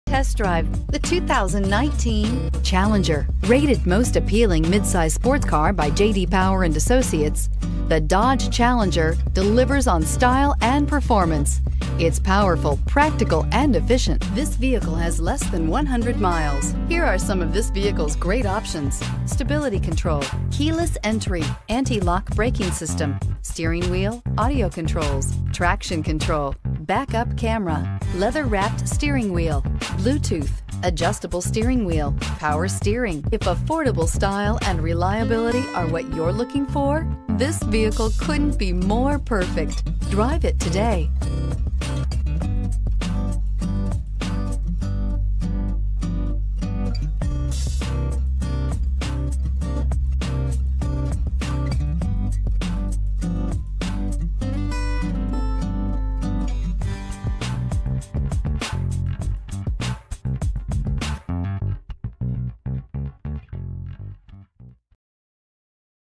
2019 Dodge Challenger Peoria AZ K1244
2019 Dodge Challenger R/T

Scores 25 Highway MPG and 16 City MPG! This Dodge Challenger delivers a Regular Unleaded V-8 5.7 L/345 engine powering this Automatic transmission. WHEELS: 20 X 8.0 GRANITE CRYSTAL ALUMINUM (STD), UCONNECT 4C NAV W/8.4 DISPLAY -inc: SiriusXM Travel Link, 5-Year SiriusXM Travel Link Service, 5-Year SiriusXM Traffic Service, 1-YR SiriusXM Guardian Trial, SiriusXM Traffic Plus, Integrated Center Stack Radio, Dodge Performance Pages, GPS Navigation, TRANSMISSION: 8-SPEED AUTOMATIC (8HP70) -inc: Auto Leather Wrapped Shift Knob, 230MM Rear Axle, Autostick Automatic Transmission, Steering Wheel Mounted Shift Control, Tip Start.* This Dodge Challenger Features the Following Options *QUICK ORDER PACKAGE 28H R/T -inc: Engine: 5.7L V8 HEMI MDS VVT, Transmission: 8-Speed Automatic (8HP70) , TIRES: P245/45ZR20 AS PERFORMANCE (STD), SIRIUSXM SATELLITE RADIO -inc: For More Info, Call , 1-Yr SiriusXM Radio Service, OCTANE RED PEARLCOAT, ENGINE: 5.7L V8 HEMI MDS VVT, BLACK, HOUNDSTOOTH CLOTH PERFORMANCE SEATS, ALPINE AUDIO GROUP W/SUBWOOFER -inc: 506 Watt Amplifier, 9 Amplified Speakers w/Subwoofer, Surround Sound, Window Grid Antenna, Voice Recorder, Variable Intermittent Wipers.* Stop By Today *Test drive this must-see, must-drive, must-own beauty today at Larry H Miller Dodge Peoria, 8665 West Bell Rd, Peoria, AZ 85382.